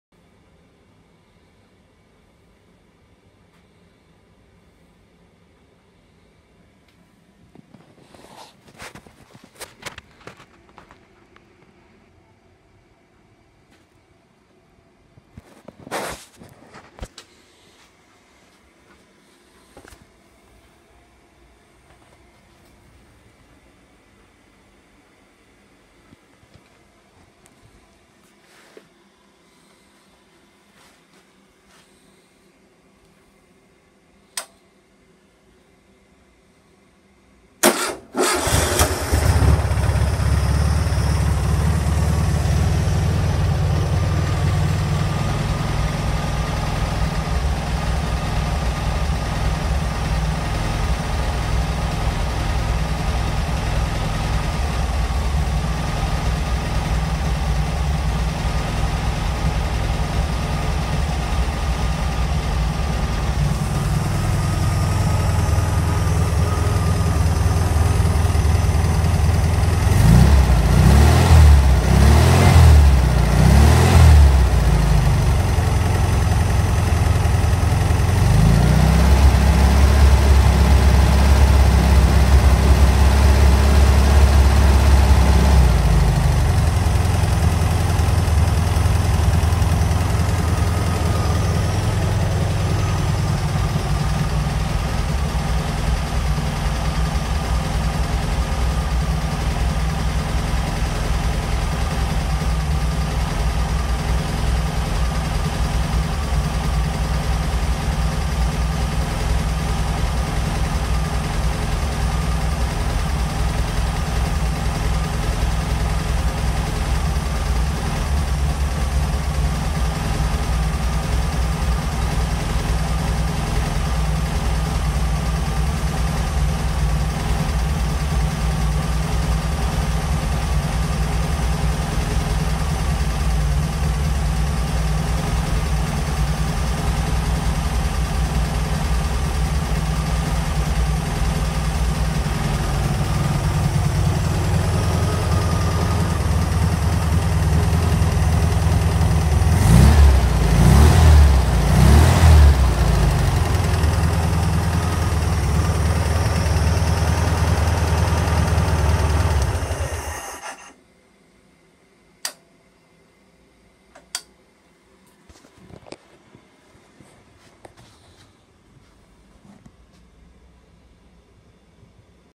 Les’s VW Beetle Flat Four
VW Beetle project flat four engine running on a test rig out of the car.
Engine is standard other than being fitted with vintage speed stainless steel rear box, electronic ignition points and EPMI stainless rocker covers.

Test rig is the rear part on a gearbox with starter motor and wired up to run with an alternator, changing the battery of the test rig.

Protect soon to be for sale on EBAY!